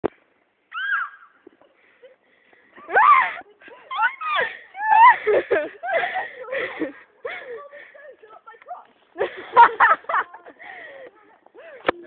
Tray sledding in the snow!
got some of the trays from the cantine and decided to use them as sleys brilliant fun!